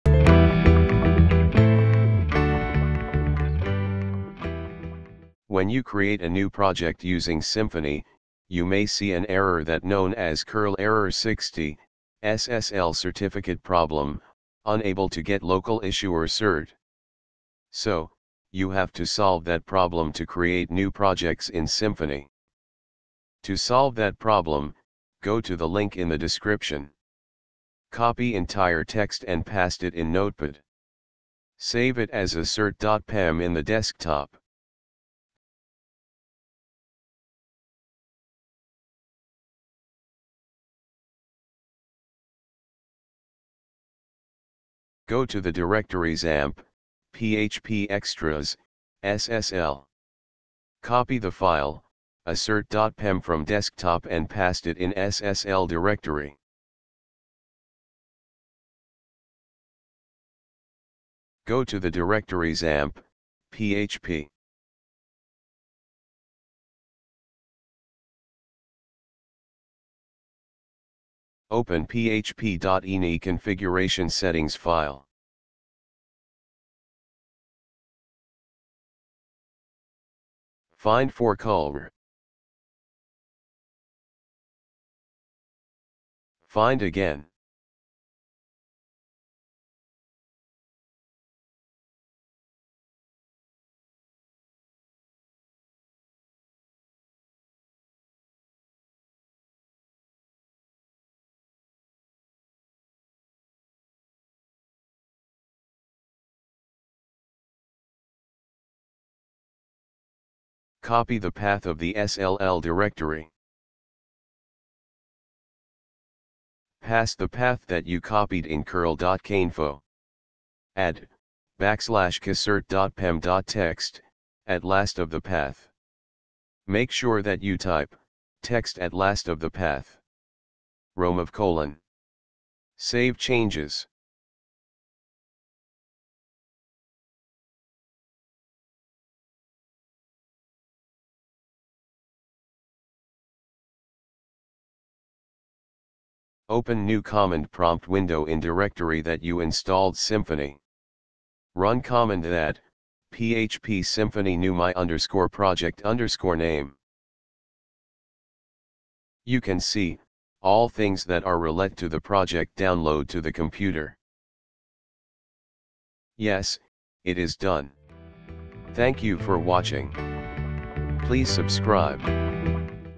how to solve cURL error 60: SSL certificate problem: unable to get local issuer cert
This tutorial guides you to resolve url error 60 ssl certificate problem in the XAMPP server installed on the Windows operating system. The given solution might depend on the operating system, PHP version, and PHP server you are using.